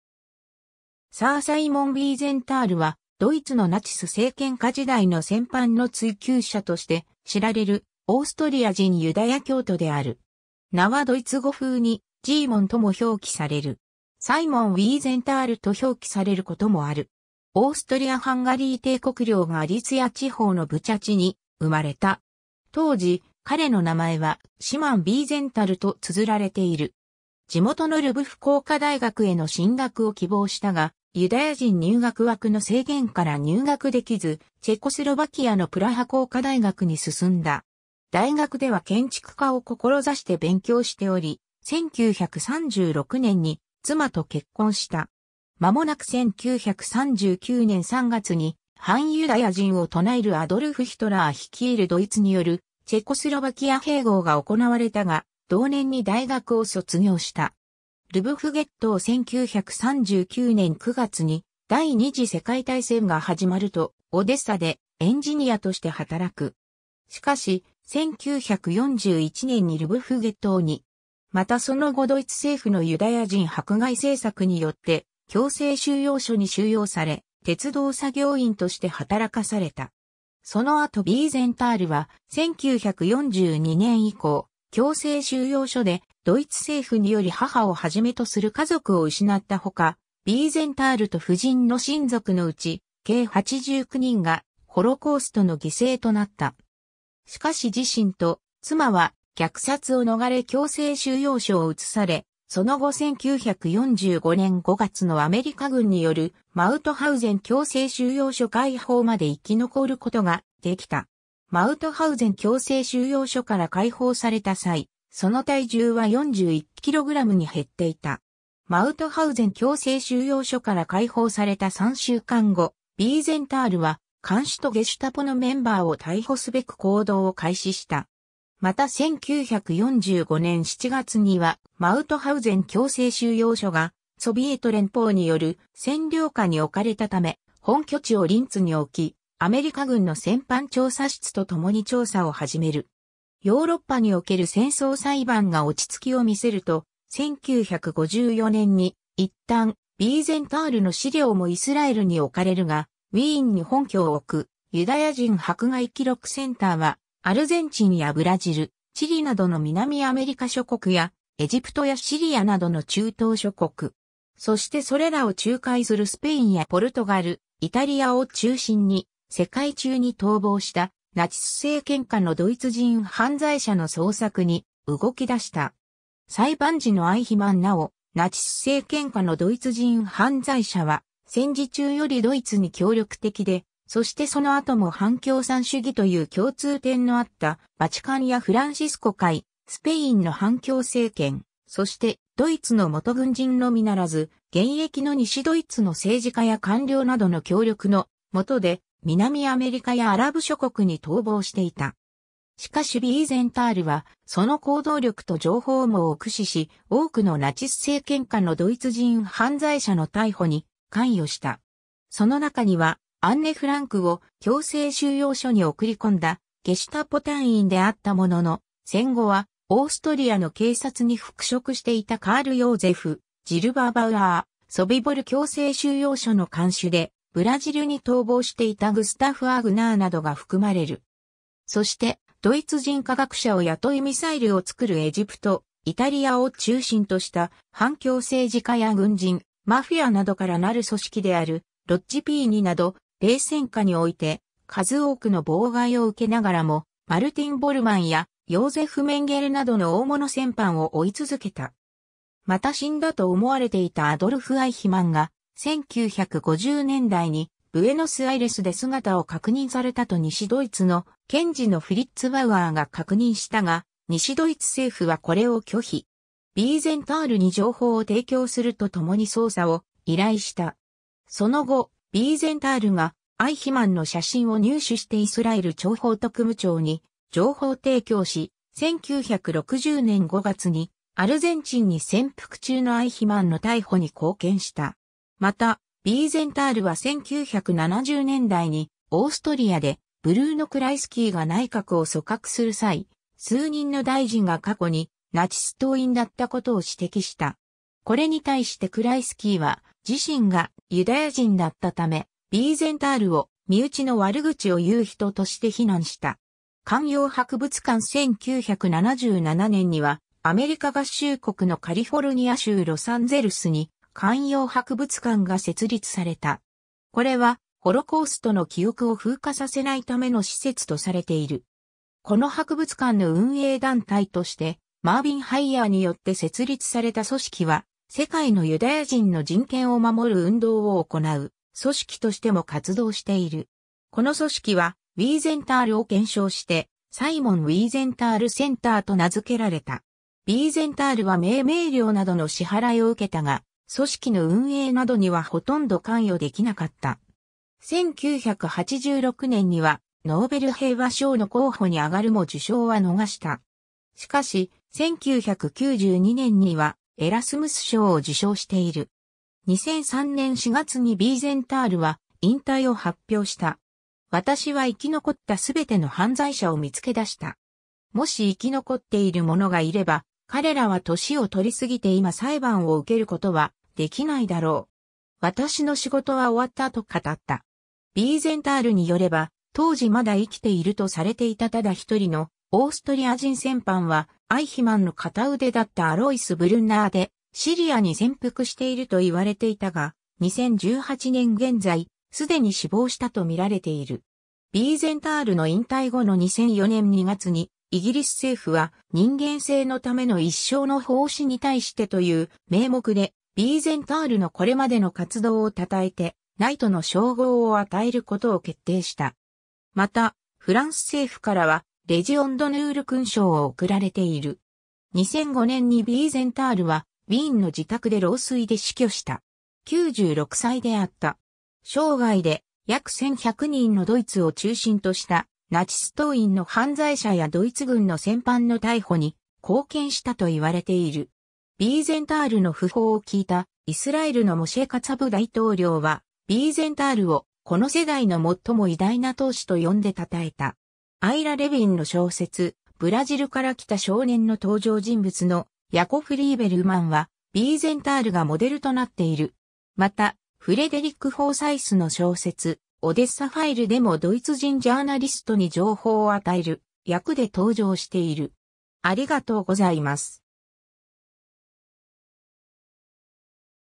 サイモン・ヴィーゼンタール
サー・サイモン・ヴィーゼンタール（Sir Simon Wiesenthal、1908年12月31日 - 2005年9月20日）は、ドイツのナチス政権下時代の戦犯の追及者（ナチ・ハンター）として知られるオーストリア人ユダヤ教徒である。
名はドイツ語風に「ジーモン」とも表記される。
サイモン・ウィーゼンタールと表記されることもある。
 オーストリア・ハンガリー帝国領ガリツィア地方のブチャチに生まれた。
当時、彼の名前は Szymon Wiesenthal（シモン・ヴィーゼンタール）と綴られている。
地元のルヴフ工科大学への進学を希望したが、ユダヤ人入学枠の制限から入学できず、チェコスロバキアのプラハ工科大学に進んだ。
 大学では建築家を志して勉強しており、1936年に妻と結婚した。
まもなく1939年3月に反ユダヤ人を唱えるアドルフ・ヒトラー率いるドイツによるチェコスロバキア併合が行われたが、同年に大学を卒業した。
 ルヴフ・ゲットー(1942年春) 1939年9月に第二次世界大戦が始まると、オデッサでエンジニアとして働く。
しかし、1941年にルヴフ・ゲットーに。
またその後ドイツ政府のユダヤ人迫害政策によって強制収容所に収容され、鉄道作業員として働かされた。
 その後ヴィーゼンタールは、1942年以降強制収容所でドイツ政府により母を始めとする家族を失った他、ヴィーゼンタールと夫人の親族のうち計89人がホロコーストの犠牲となった。
しかし自身と妻は虐殺を逃れ強制収容所を移され、その後1945年5月のアメリカ軍による、マウトハウゼン強制収容所解放まで生き残ることができた。
マウトハウゼン強制収容所から解放された際、その体重は41キログラムに減っていた。
 マウトハウゼン強制収容所から解放された3週間後、ヴィーゼンタールは看守とゲシュタポのメンバーを逮捕すべく行動を開始した。
また1945年7月にはマウトハウゼン強制収容所がソビエト連邦による占領下に置かれたため、本拠地をリンツに置きアメリカ軍の戦犯調査室とともに調査を始める。
 ヨーロッパにおける戦争裁判が落ち着きを見せると、1954年に一旦ヴィーゼンタールの資料もイスラエルに置かれるが、ウィーンに本拠を置くユダヤ人迫害記録センターは、アルゼンチンやブラジル、チリなどの南アメリカ諸国や、エジプトやシリアなどの中東諸国。
そしてそれらを仲介するスペインやポルトガル、イタリアを中心に、世界中に逃亡したナチス政権下のドイツ人犯罪者の捜索に動き出した。
 裁判時のアイヒマン なおナチス政権下のドイツ人犯罪者は、戦時中よりドイツに協力的で、そしてその後も反共産主義という共通点のあったバチカンやフランシスコ会、スペインの反共政権、そしてドイツの元軍人のみならず、現役の西ドイツの政治家や官僚などの協力の下で南アメリカやアラブ諸国に逃亡していた。
 しかしヴィーゼンタールはその行動力と情報網を駆使し、多くのナチス政権下のドイツ人犯罪者の逮捕に関与した。
その中にはアンネ・フランクを強制収容所に送り込んだゲシュタポ隊員であったものの、戦後はオーストリアの警察に復職していたカール・ヨーゼフ・ジルバーバウアー、ソビボル強制収容所の看守でブラジルに逃亡していたグスタフ・ワーグナーなどが含まれる。
 そしてドイツ人科学者を雇いミサイルを作るエジプト、イタリアを中心とした反共政治家や軍人、マフィアなどからなる組織である「ロッジP2」など、冷戦下において数多くの妨害を受けながらも、マルティン・ボルマンやヨーゼフ・メンゲレなどの大物戦犯を追い続けた。
 また死んだと思われていたアドルフ・アイヒマンが、1950年代にブエノスアイレスで姿を確認されたと西ドイツの検事のフリッツ・バウアーが確認したが、西ドイツ政府はこれを拒否。
ヴィーゼンタールに情報を提供するとともに捜査を依頼した。
その後、ヴィーゼンタールがアイヒマンの写真を入手してイスラエル諜報特務庁に情報提供し、1960年5月にアルゼンチンに潜伏中のアイヒマンの逮捕に貢献した。
 また、ヴィーゼンタールは1970年代にオーストリアでブルーノ・クライスキーが内閣を組閣する際、数人の大臣が過去にナチス党員だったことを指摘した。
これに対してクライス...